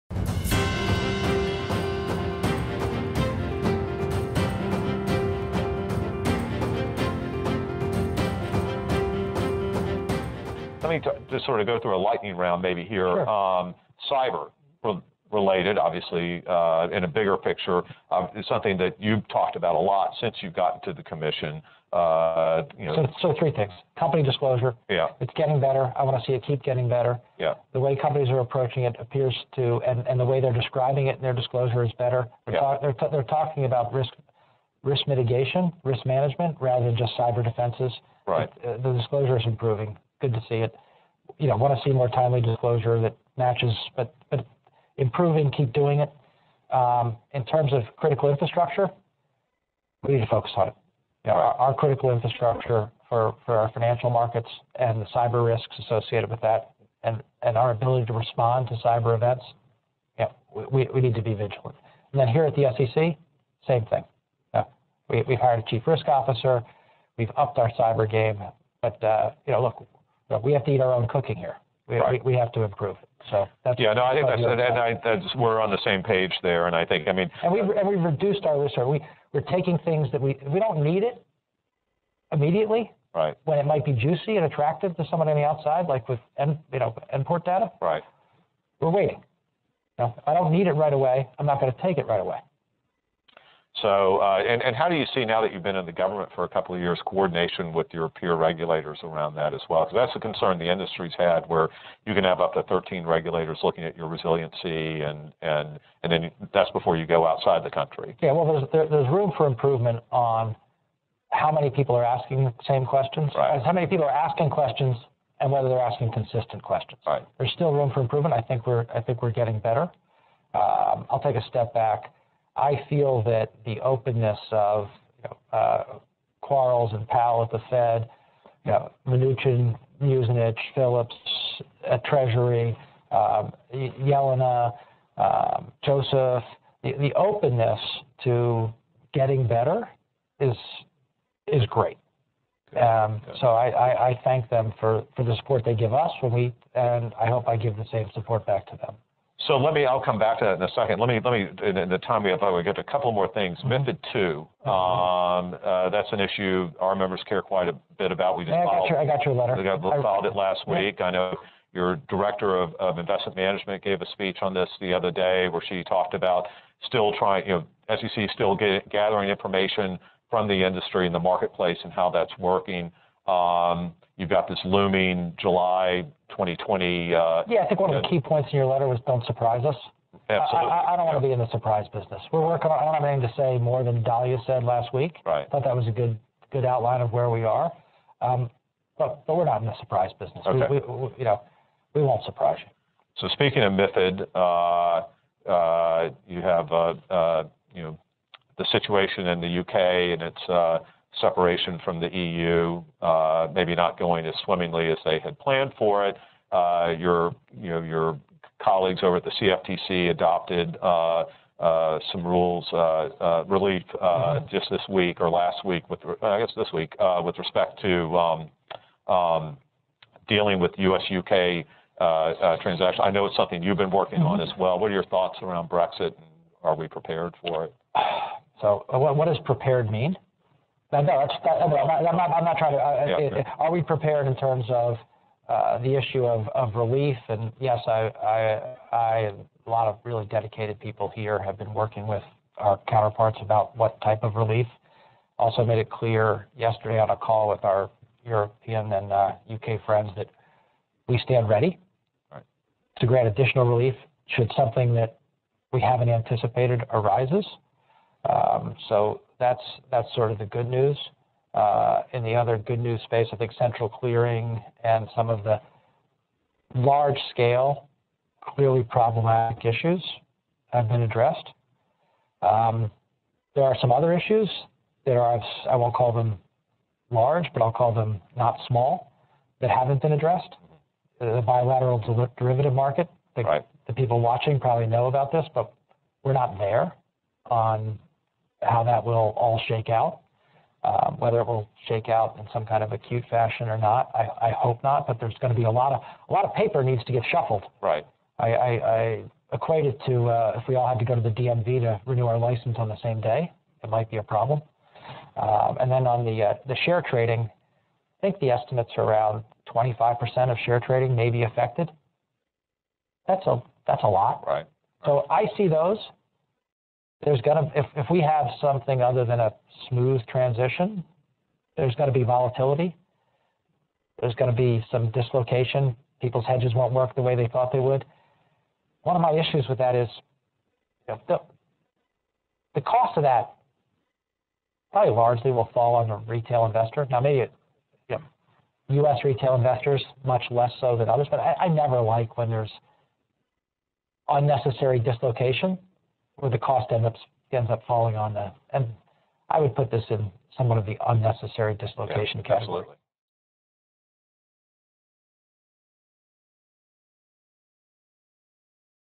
Lightning Round: Cyber, Harmonization, MiFID II, Brexit
SEC Chairman Jay Clayton and SIFMA President and CEO Kenneth E. Bentsen, Jr. discuss improving cybersecurity, including risk management, disclosure and critical infrastructure; openness to improving harmonization across regulatory agencies; the expiration of no-action relief on MiFID II; and preparing for Brexit.